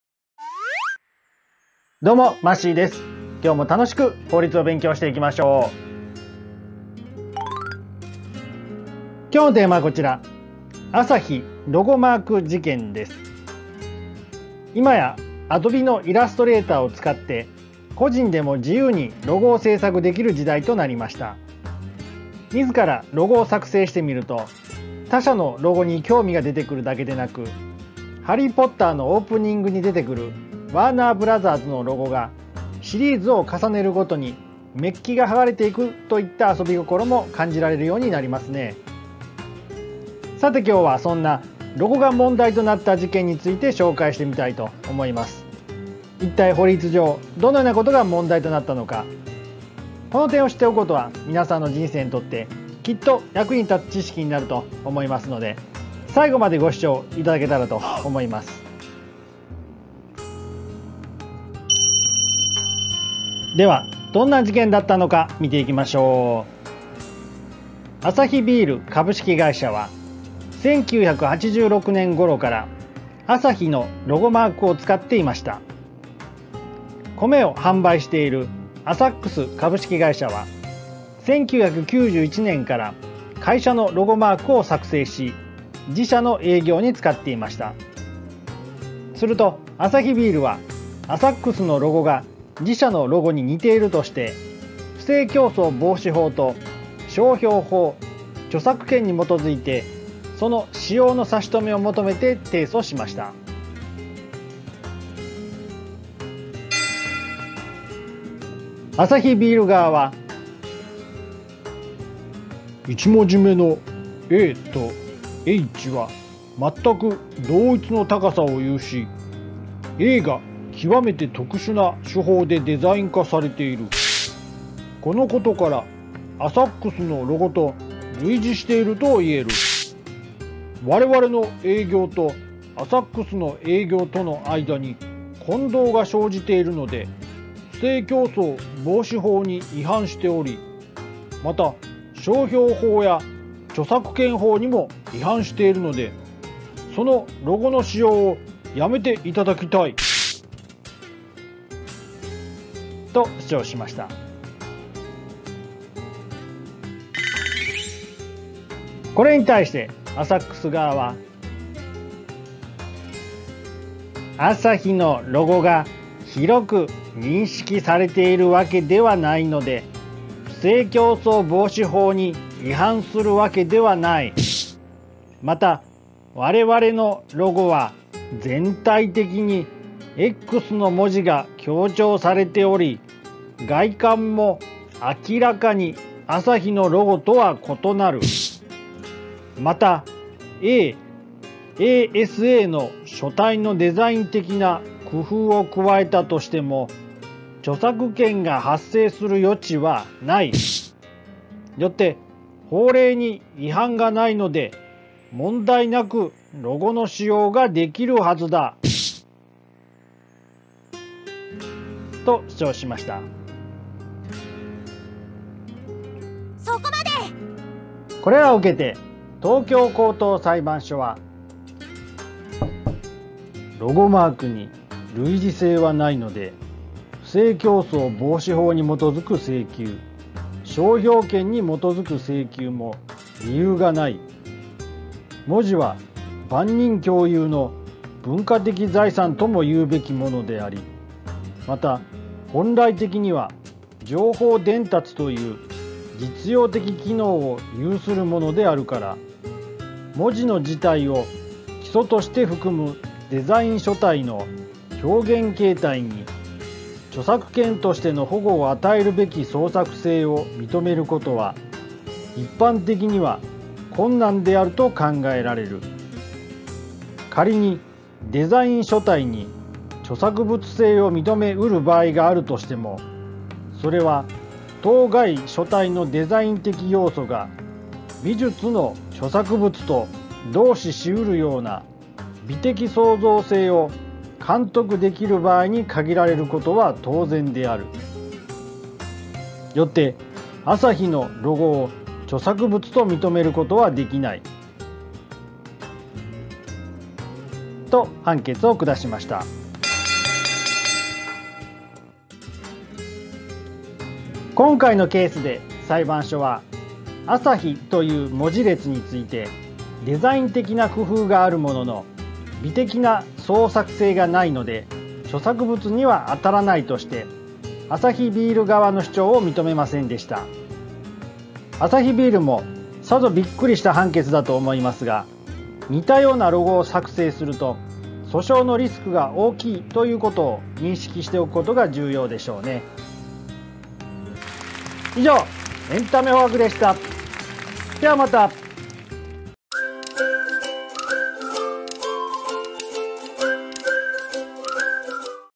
【アサヒロゴマーク事件】文字列Asahiと似たようなロゴの使用の差し止めが認められなかったのはなぜか？
「Asahi」と「AsaX」のロゴが似ていることについて、裁判所が「Asahi」という文字列について、デザイン的な工夫があるものの、美的な創作性がないので著作物にはあたらないとして、アサヒビール側の主張を認めなかった事件です。

【動画目次】
0:00  OP
1:02  どんな事件だったのか
1:48  原告の主張
2:38  被告の主張
3:37  東京高等裁判所の判決
5:03  文字列のみのロゴに著作権がないことも

東京高判平成8年1月25日についてはこちら